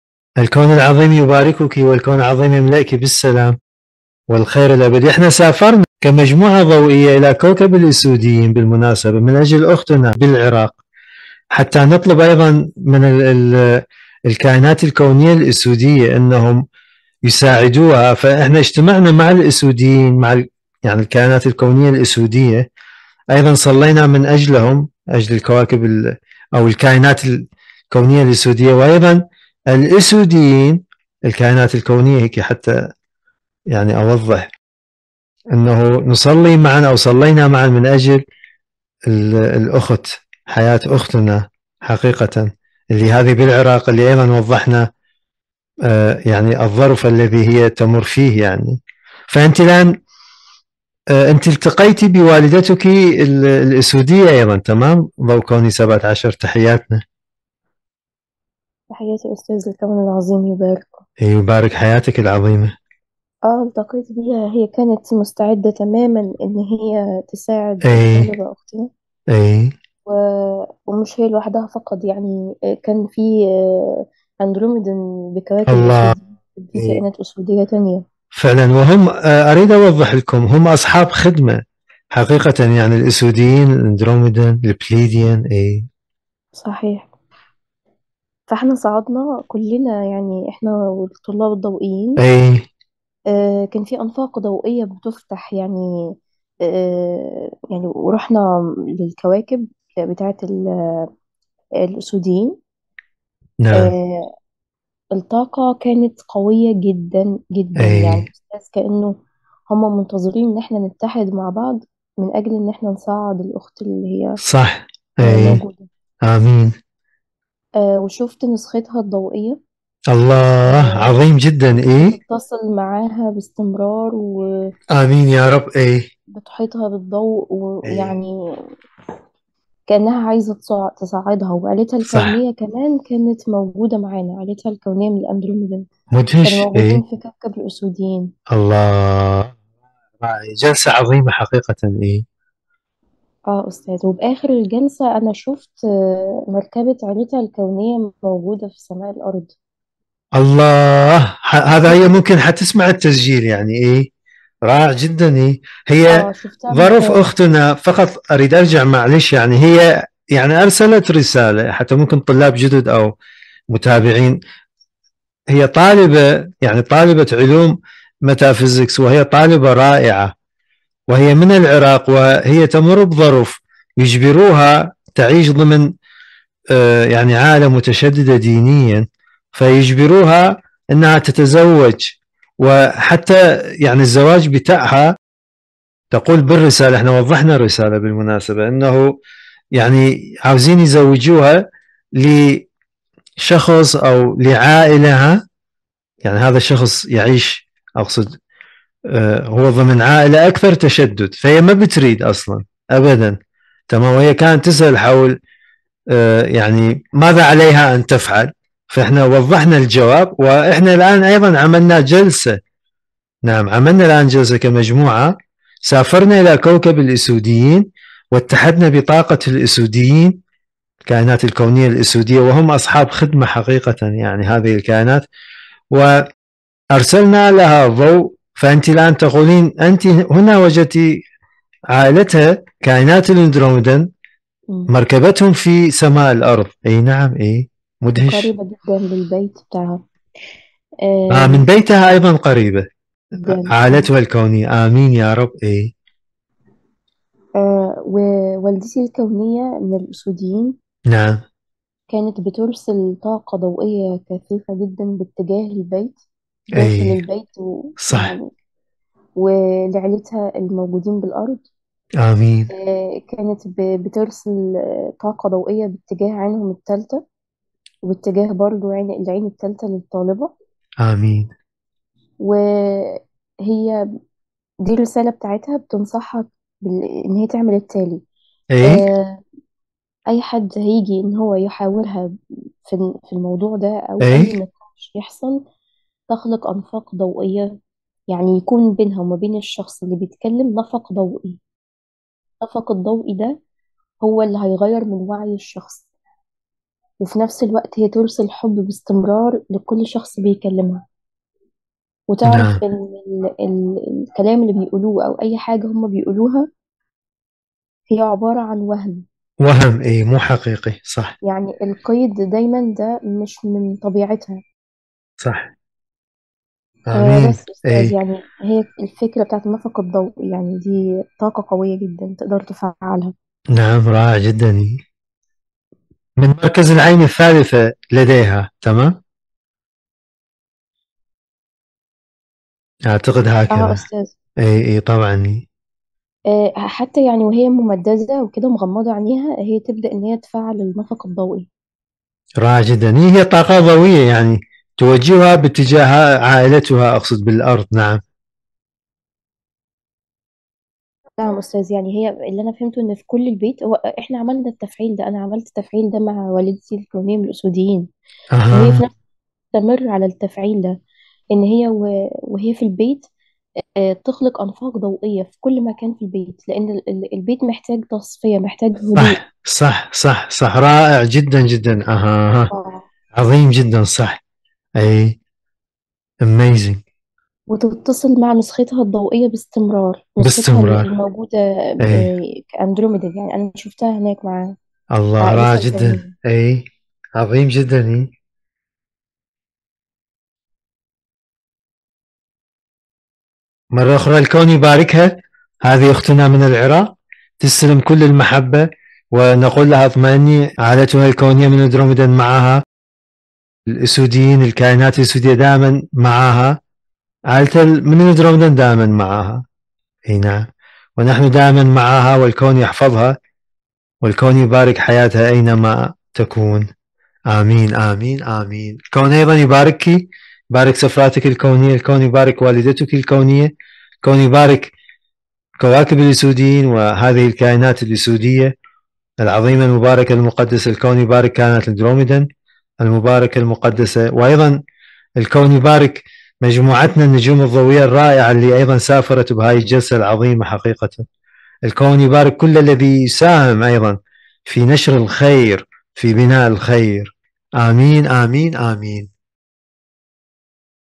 مناقشة جلسة سفر كونية الى كوكب الكائنات الاسودية نسالهم ان يساعدوا اخت بالعراق
قائمة مشاهدة .. مصادر  علوم السفر الكوني والكائنات الكونية